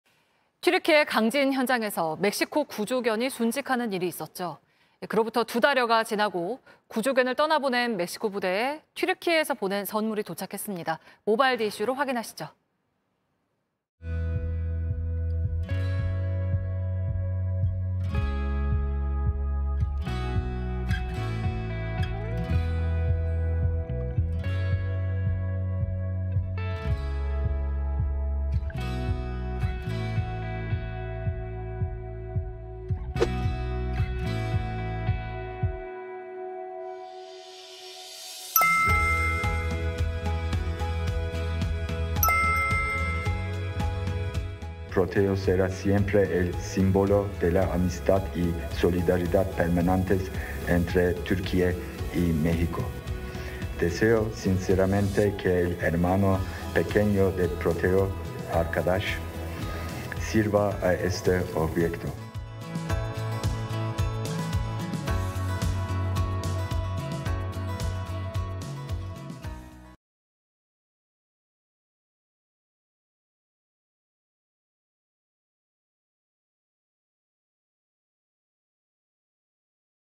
[D:이슈] 멕시코에 새 구조견 보낸 튀르키예…이름은 '친구' / JTBC 뉴스룸
튀르키예 강진 현장에서 멕시코 구조견이 순직하는 일이 있었죠. 그로부터 두달여가 지나고 구조견을 떠나보낸 멕시코 부대에 튀르키예에서 보낸 선물이 도착했습니다. 모바일 D:이슈로 확인하시죠.

지난 2월 튀르키예 강타한
대지진 당시 파견됐던

멕시코 구조견 가운데
'프로테오'

잔해 속 29명 시신 찾고
수색 중에 순직

장례식서 프로테오에 경의 표한 사람들

그후 튀르키예 당국
새로운 구조견 멕시코에 선물

이름은 '아르카다스'
튀르키예어로 '친구'

[일한 투그/멕시코 주재 튀르키예 대사 : 프로테오의 동생 아르카다스가 이 정부를 위해 일할 수 있기를 바랍니다. 프로테오는 항상 멕시코와 튀르키예 간의 영구적인 우정과 연대의 상징이 될 것입니다.]

멕시코 부대에 합류한
든든한 '댕댕이'

(출처 : 멕시코 국방부)